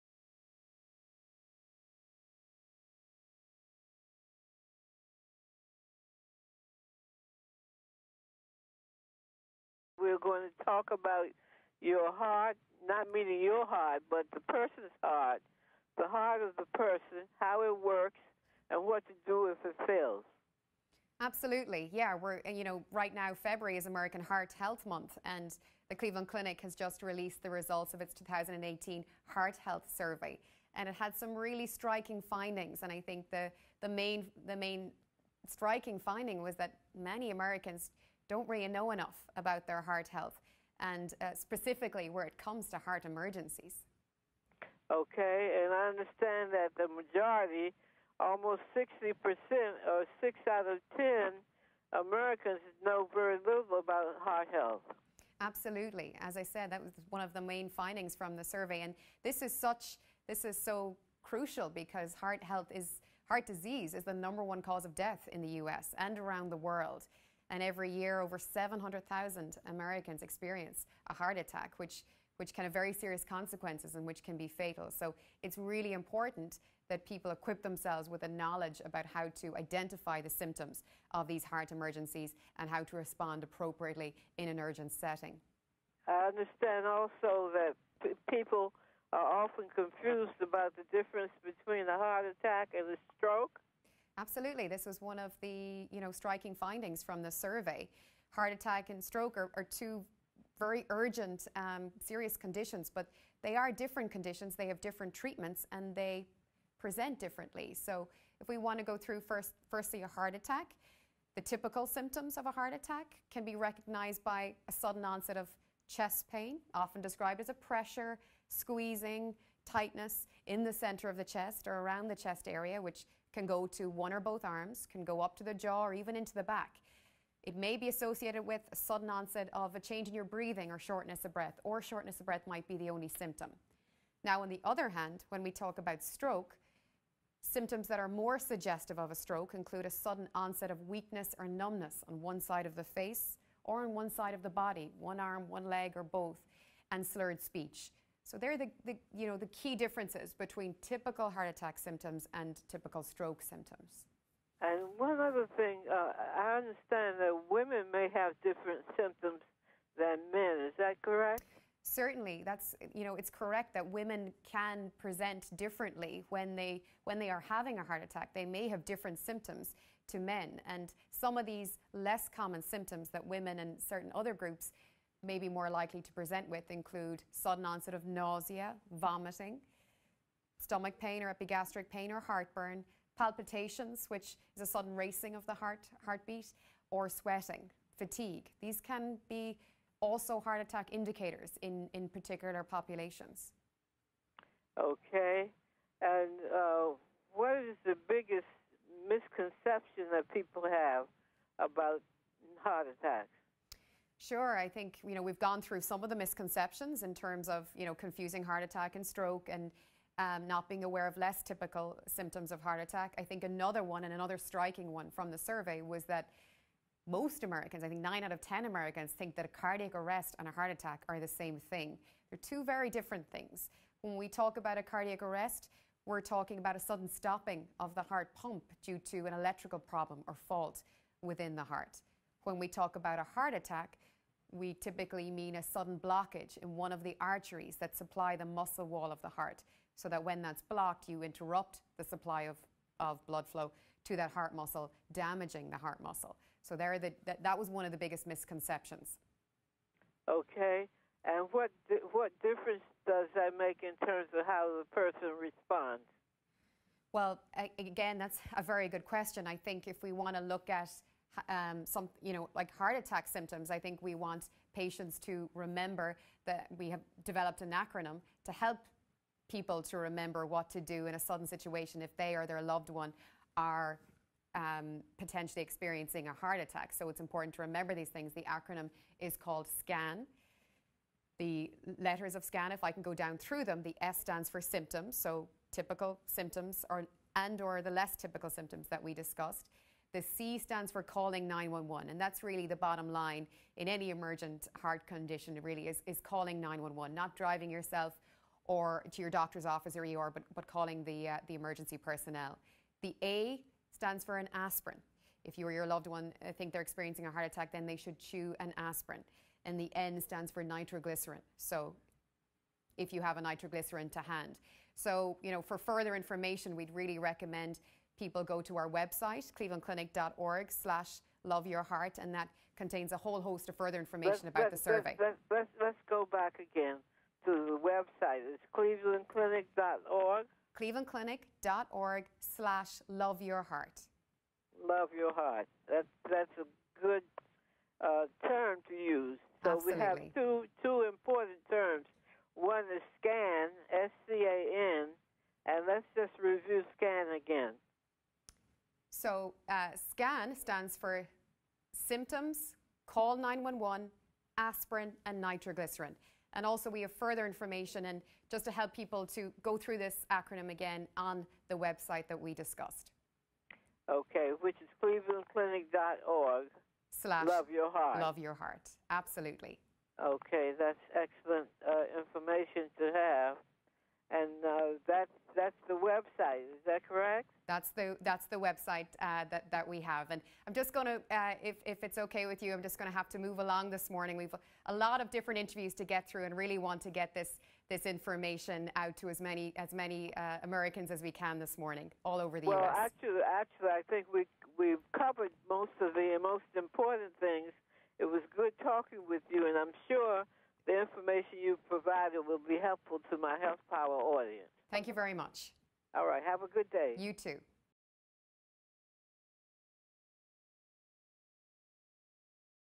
Your Heart: How It Works and What To Do If It Fails
Dr. Norma Goodwin, President of Health Power for Minorities, interviews Dr. Emer Joyce, a heart failure specialist at the Cardio-vascular Department at the Cleveland Clinic. How the heart works and what to do in an emergency was a very timely subject for discussion because February is American Heart Month.  Dr. Joyce reviewed key findings from the Cleveland Clinic 2018 Heart Health Survey.

A key survey finding was that most  (59%) Americans know little or nothing about heart health overall.  Life-saving steps to take if one has a heart attack were reviewed based on the Cleveland Clinic S.C.A.N. recommendations, and more information was provided through the Cleveland Clinic website.